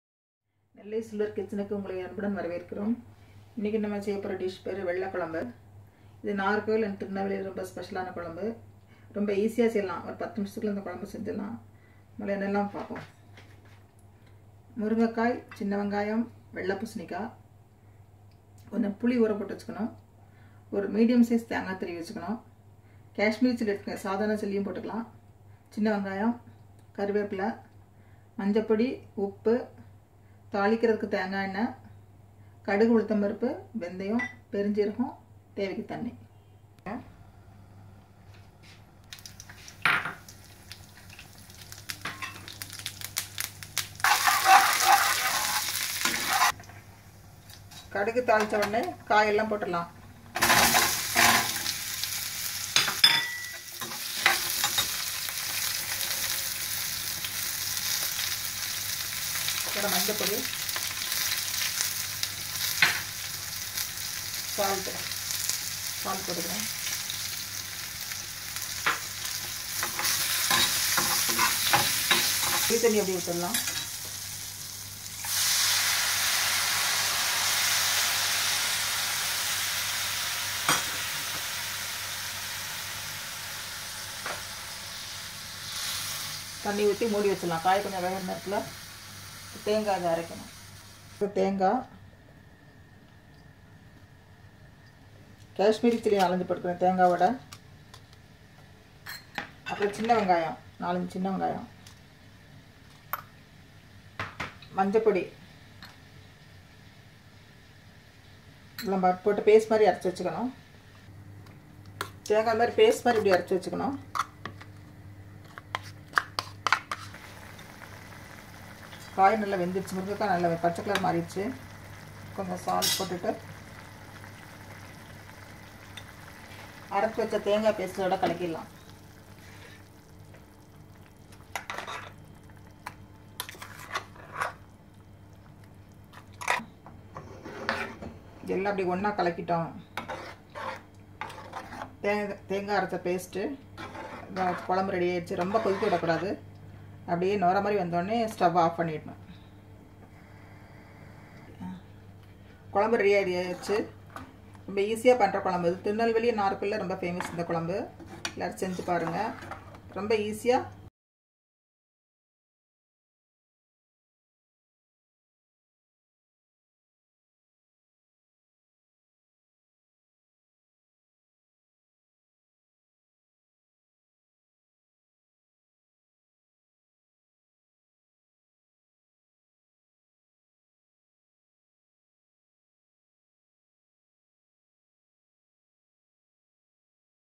How to make Vellai kuzhambo quick tasty by Nellai Silver Kitchen
Nellai Silver kitchen presents Vellai Kuzhambo
Vellai Kuzhambo is a south Indian Nellai Nanjil vegetarian dish.
Made of coconut grinded paste , with vegtable  no dall.
Medium spicy.Easy to cook
Served with rice  yummy
View our other recipes.
Welcome your views and comments.